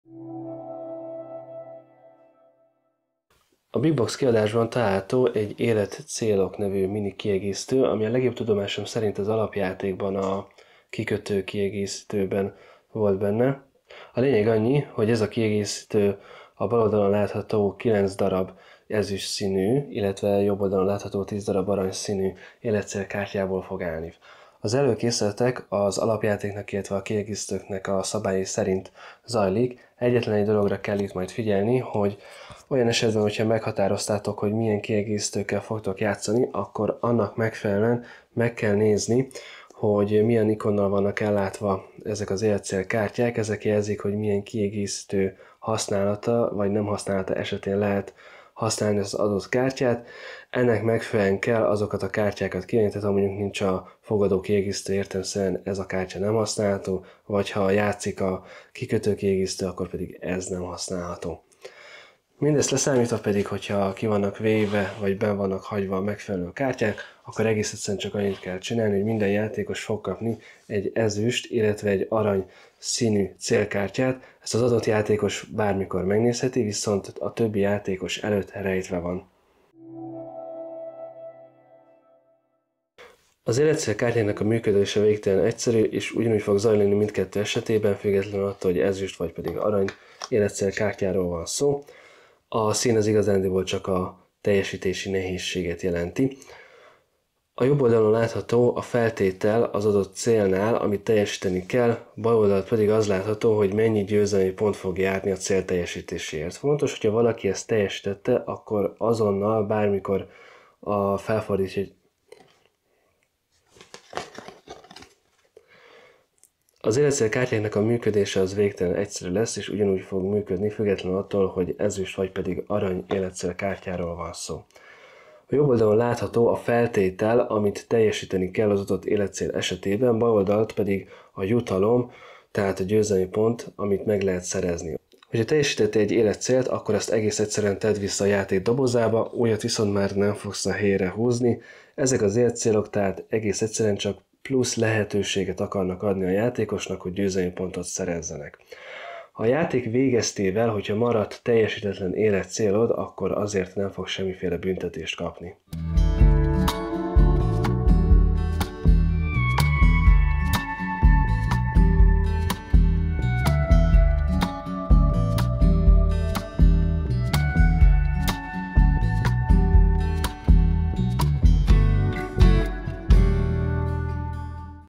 Életcélok Mini Kiegészítő | Village Big Box Tutorial 6. rész
A Village társasjáték Big Box kiadásának szabályismertetője.